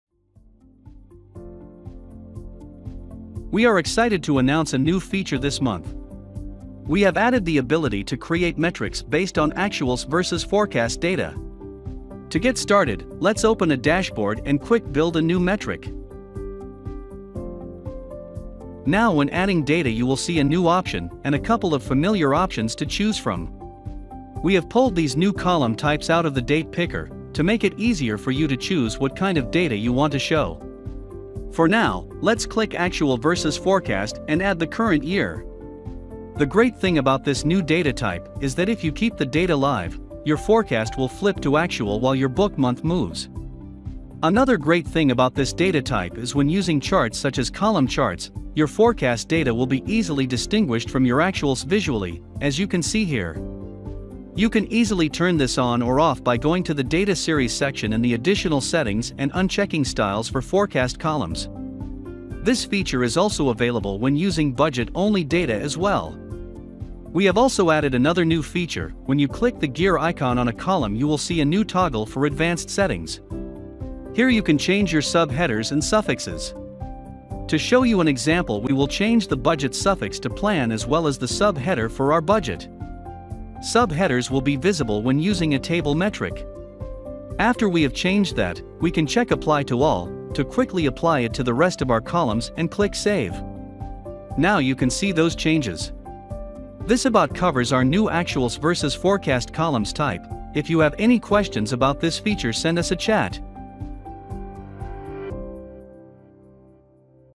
Actuals vs Budget Column Type
Learn about the new Actual v. Budget column type and how to create metrics with it.